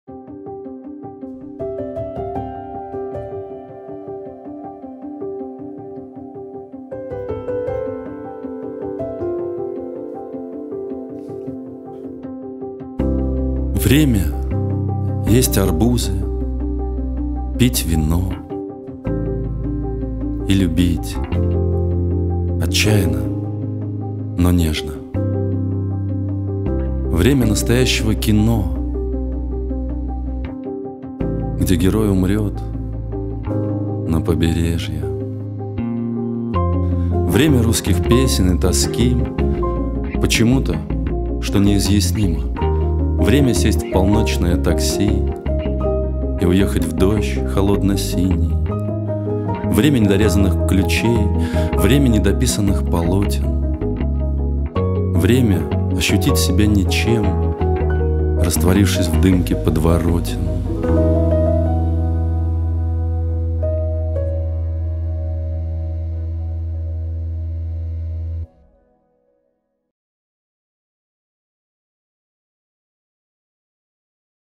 Поэзия безвременья
Музыкально-поэтический вечер «Поэзия безвременья».
20 декабря в 19.00 | Театр Пушкина

Музыкально-поэтический спектакль. Вечер светлой лирики, откровенный разговор о любви, Боге, одиночестве. Откроем душу рифмами искренности и любви под звуки волшебной музыки.

поэт и исполнитель - Алексей Анищенко,
композитор - Максим Войтов,
оператор - Дмитрий Нигматуллин

20 декабря в 19.00
Театр Пушкина